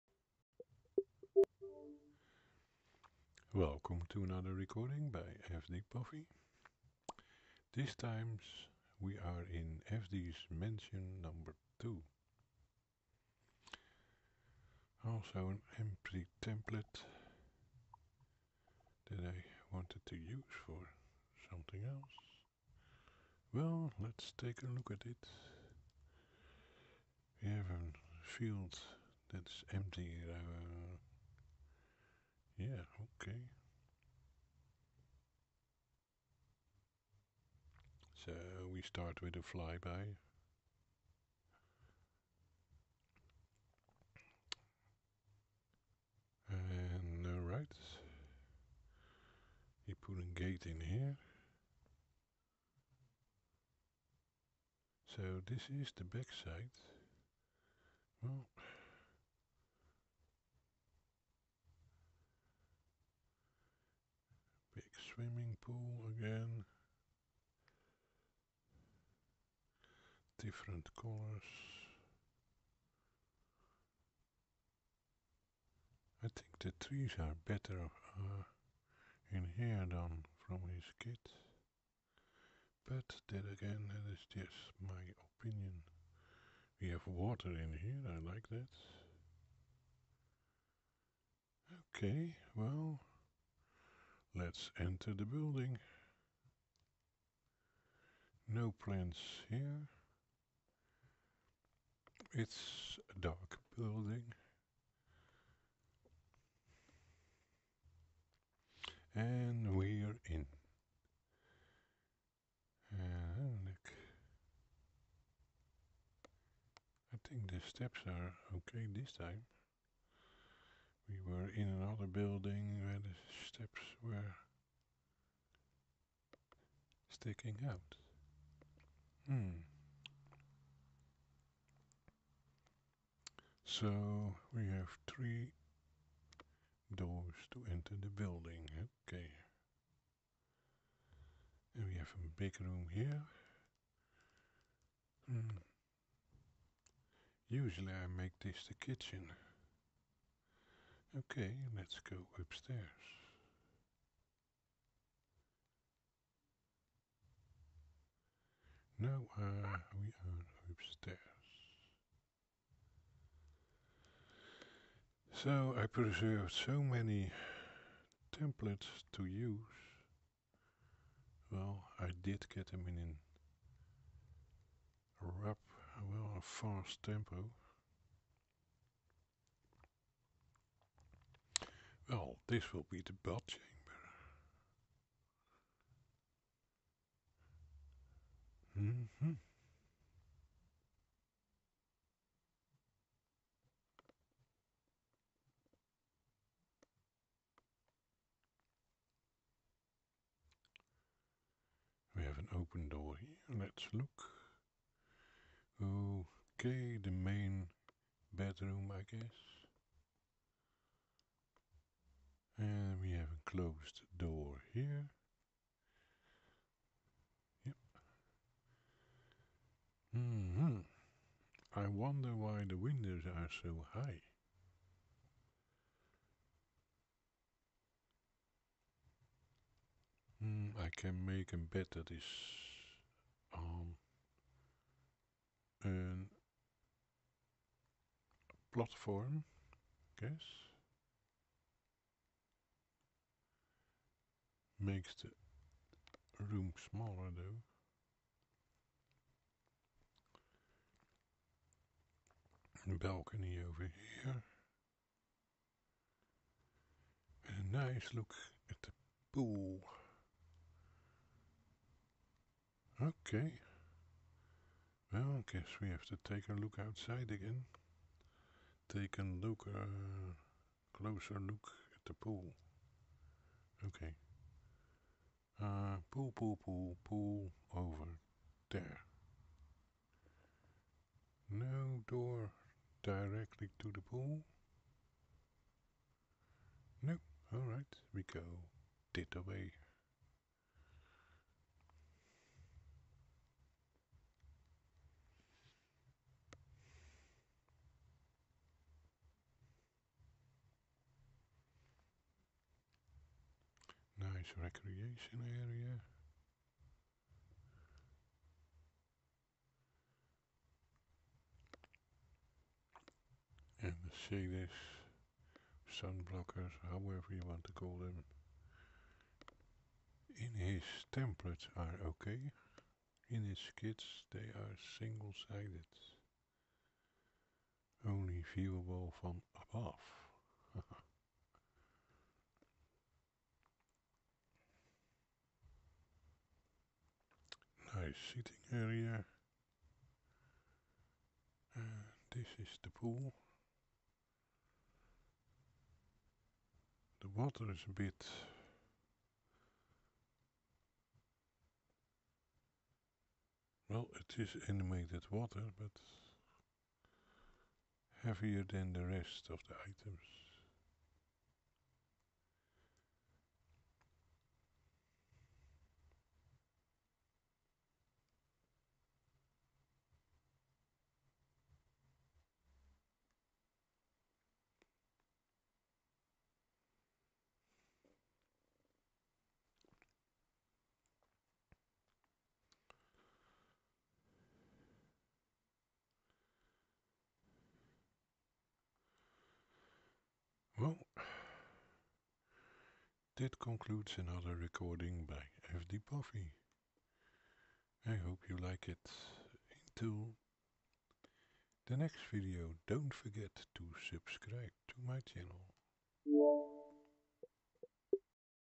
FD’s Mansion 2 Empty
Kit's and templets are starting to disappear we coming closer to the close down
Altspace was closedown by Microsoft, so i try to make memories , to preserve what once was